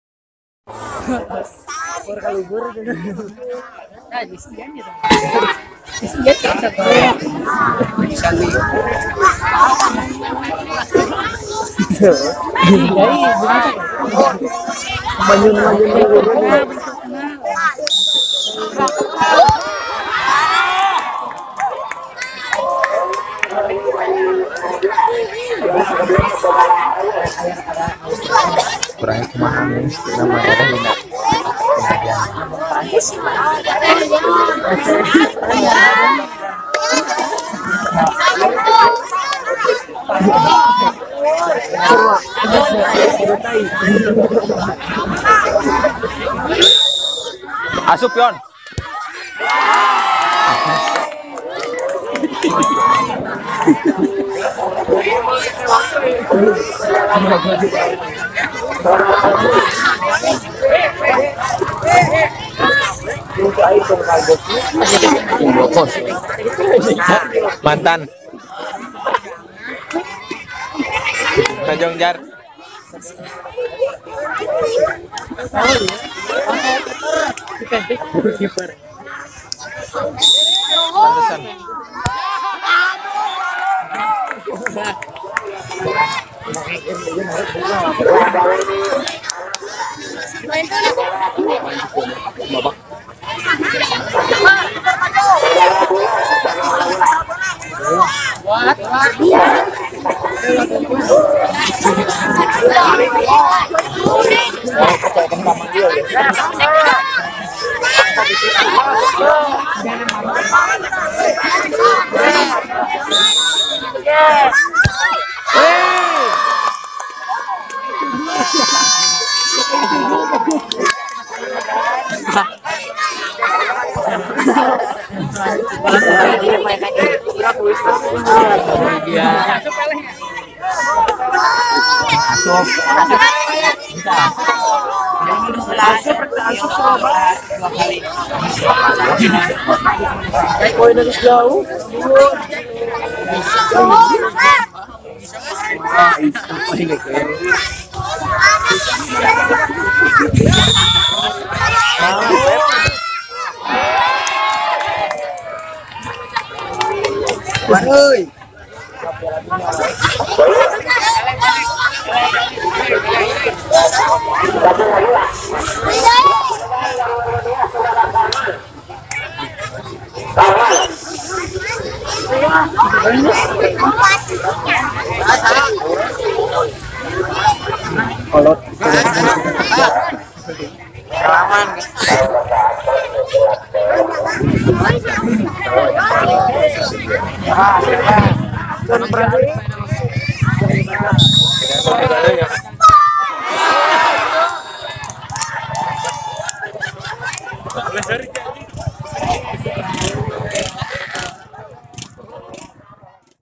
Detik Detik PENALTY KICK : (RW 02 SIRNA (A) VS RW 07 PAGADUNGAN
CIGAYAM CUP 2018

28 Juli 2018

Lapangan Bugel Desa Cigayam, Kecamatan Banjaranyar Kabupaten Ciamis